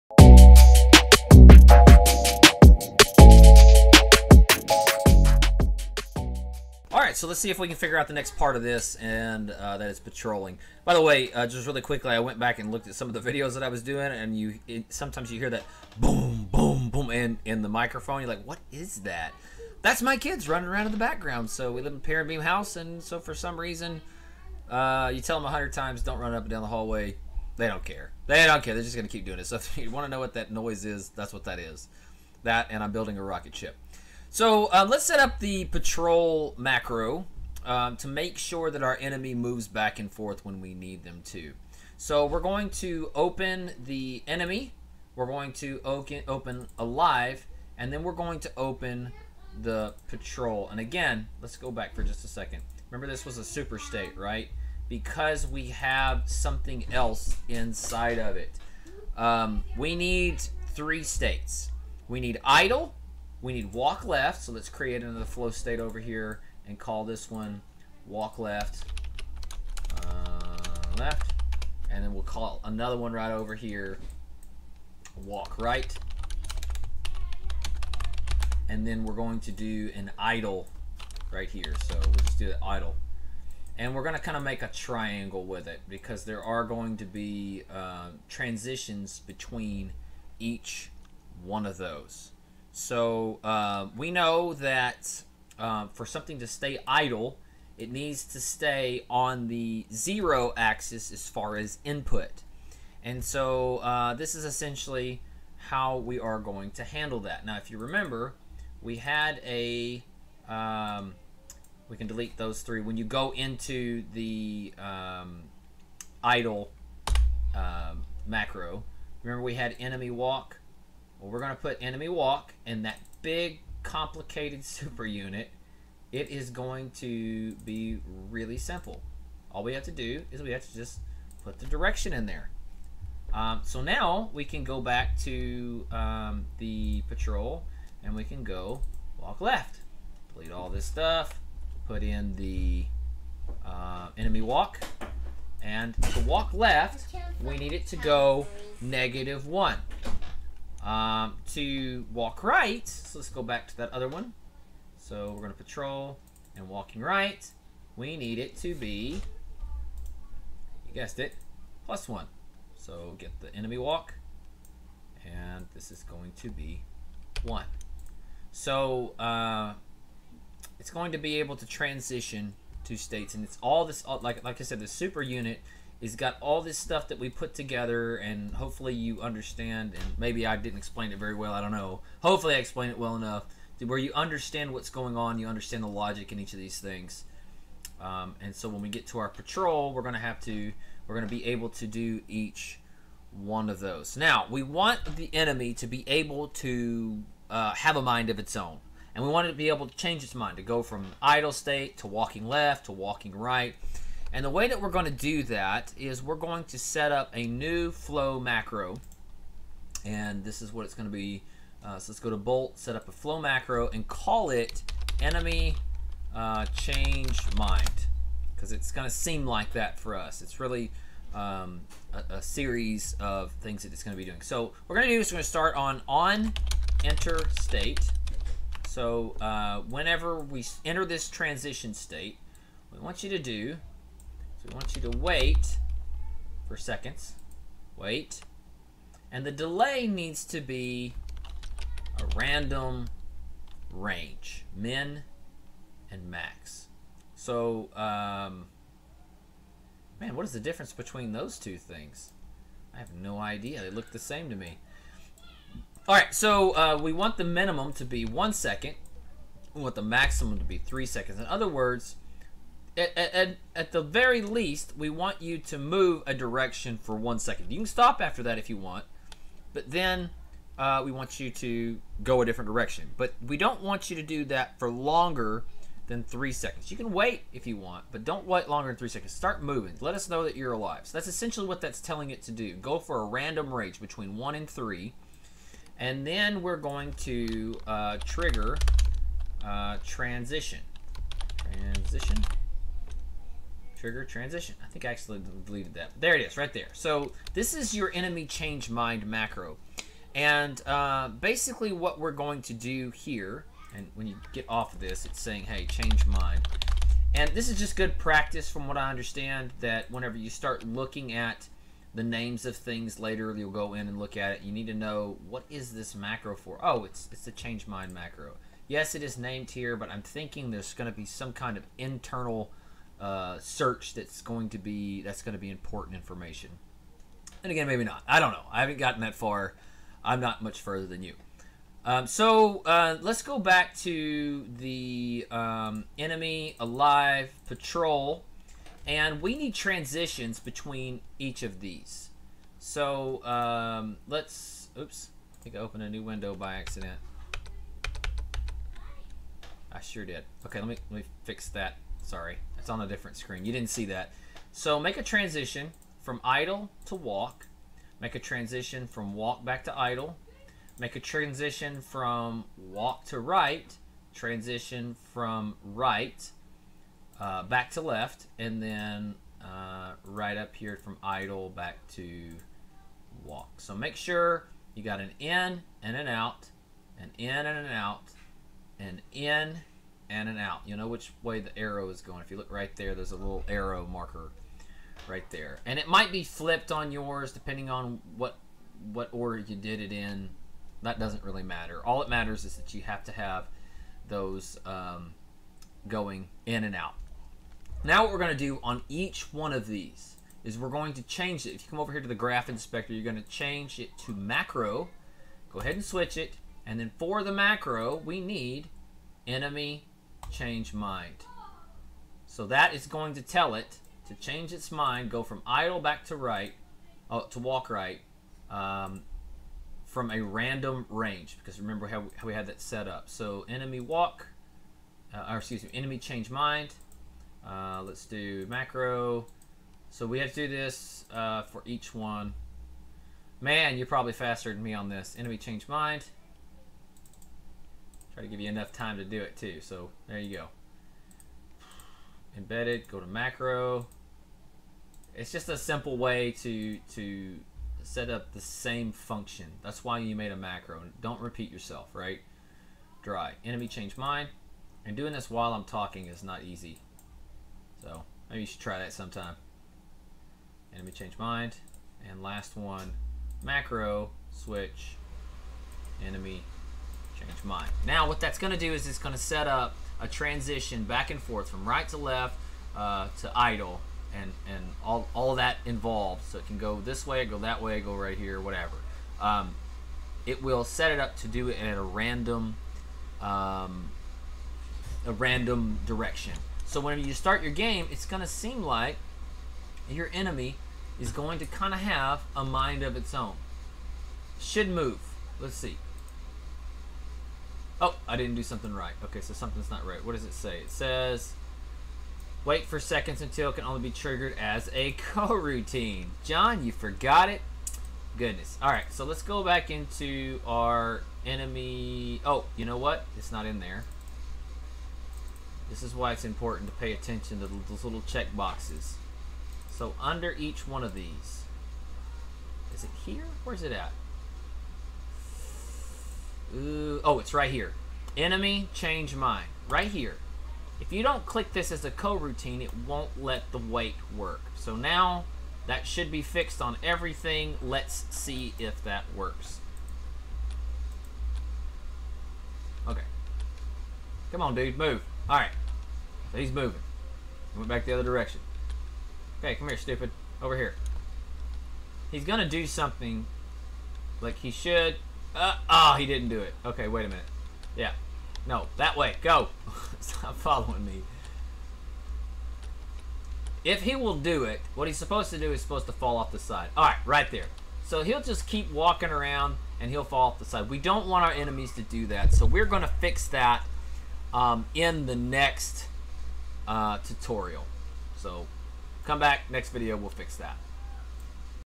Easy Free Video Game Design Tutorial # 14 - Enemy AI - Patrol - Part 1
In this video, we begin creating the Enemy AI - Patrol game logic using Unity + Bolt Visual Scripting